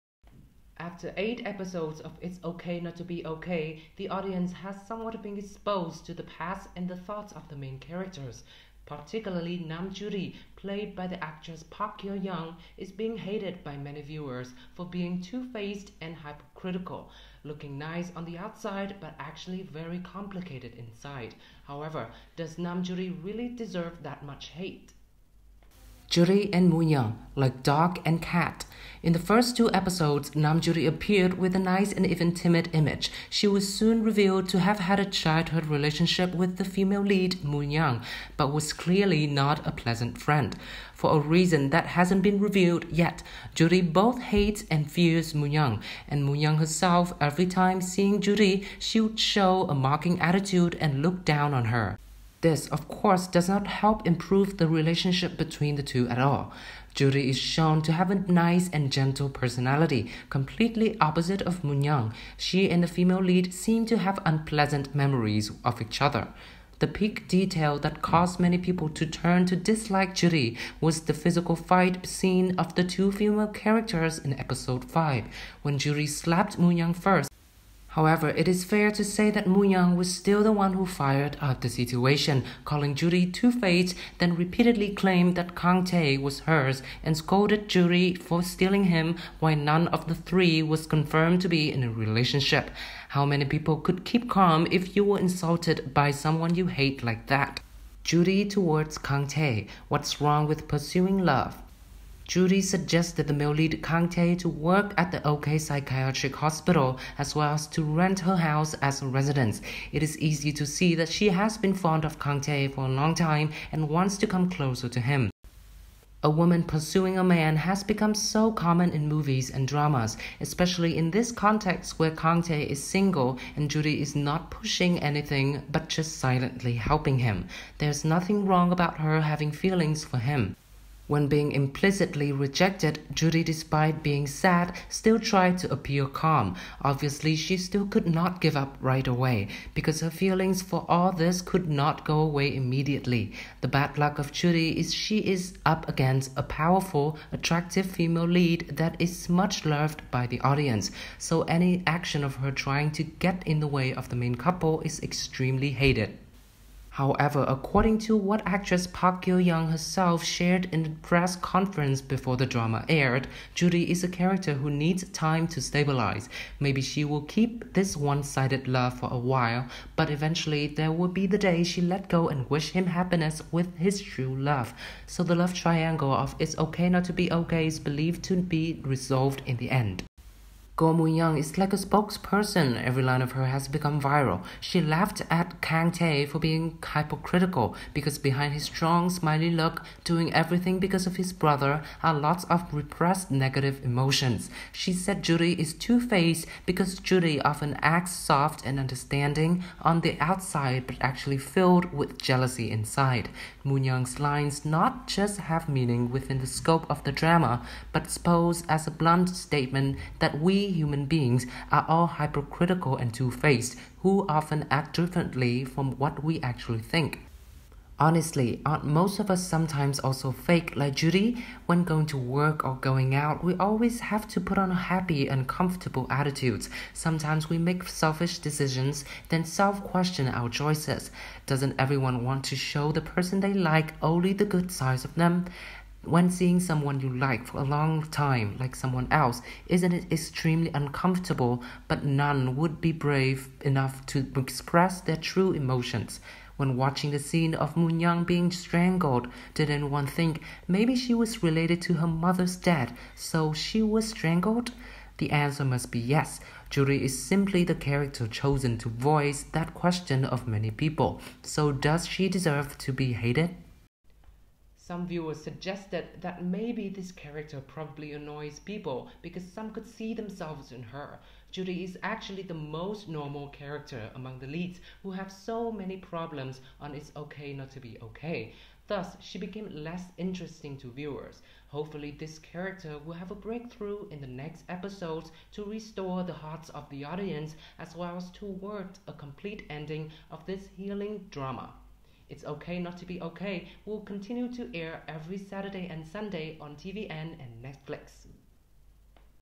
Does Nam Joo Ri really deserve to be the most hated character on It’s Okay to Not Be Okay?
Female character Nam Joo Ri has been receiving much hate from many viewers of the drama It’s Okay to Not Be Okay for being “two-faced”, but does she really deserve to be hated?